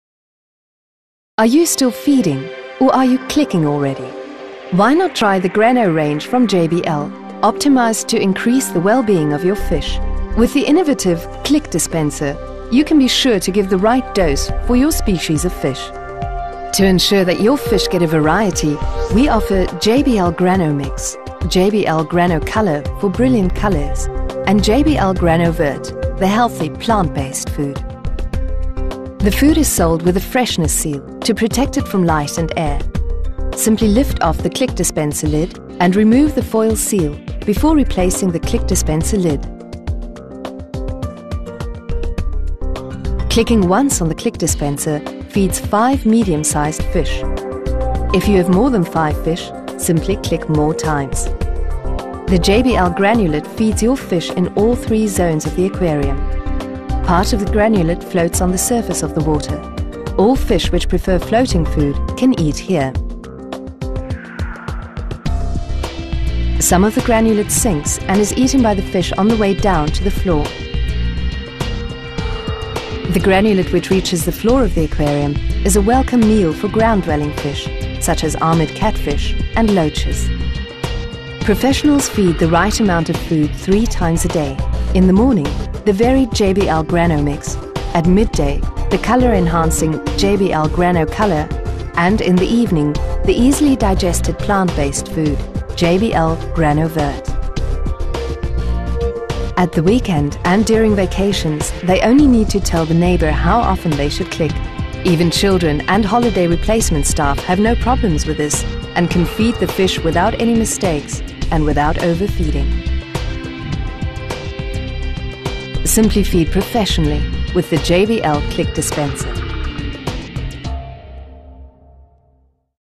JBL NovoGranoMix mini click - Nourriture pour Aquarium
Présentation et Test de la nourriture pour poissons d'aquarium JBL NovoGranoMix mini click

Livraison possible vers toute l'Europe